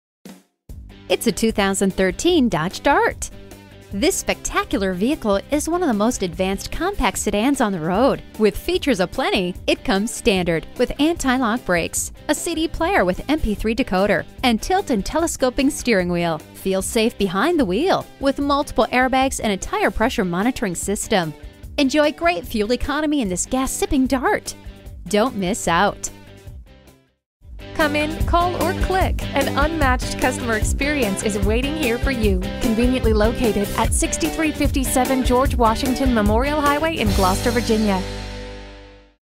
2013 Dodge Dart Gloucester VA Newport-News, VA P7064 - SOLD
Year: 2013
Make: Dodge
Model: Dart
Engine: 4 Cylinder
Color: Silver
Mileage: 34042
Stock #: P7064
Right car! Right price! Why pay more for less?! CarFax 1-Owner... When was the last time you smiled as you turned the ignition key? Feel it again with this attractive-looking 2013 Dodge Dart. You, out on the road in this superb, one-owner Dart, would look so much better than it sitting here, all sad and lonely, on our lot. It's only known one loyal master, but now it's ready to be loved again. Come get it!

Address:
6357 George Washington Memorial HWY